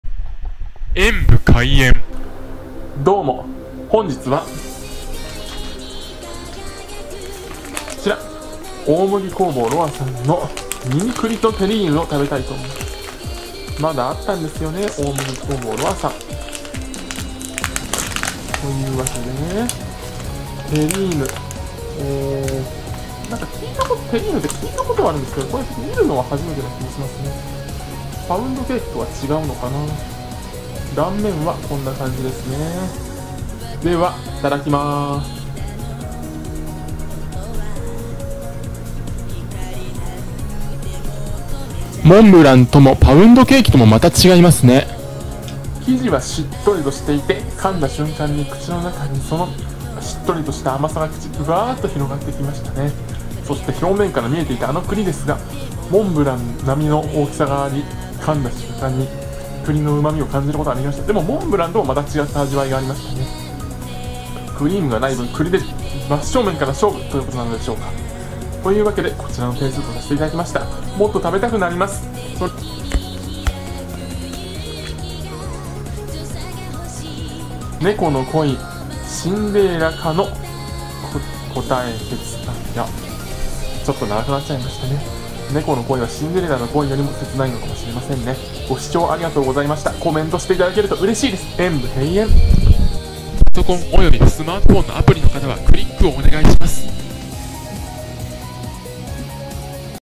大麦工房ロア 栗のテリーヌを食べてみた。モンブランともパウンドケーキとも違う～
大麦工房ロア　栗のテリーヌを食べてみました。
モンブランともパウンドケーキとも違う栗の味わい、でもテリーヌって抑々何なんだろう？
贈り物は此方まで↓
〒153-0042